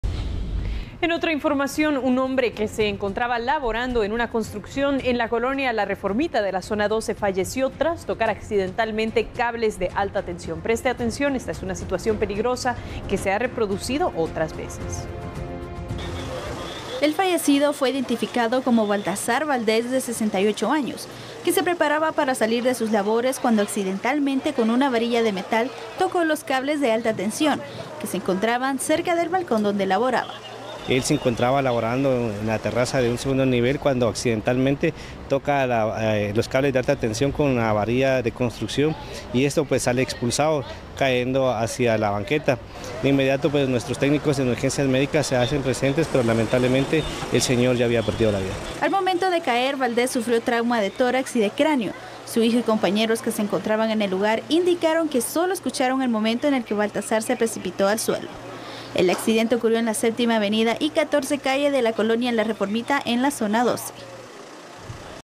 Un hombre murió electrocutado en colonia La Reformita
La víctima de 68 años se encontraba trabajando en una construcción en la colonia La Reformita de la zona 12. El hombre tocó con una varilla de matal, casualmente, los cables de alta tensión.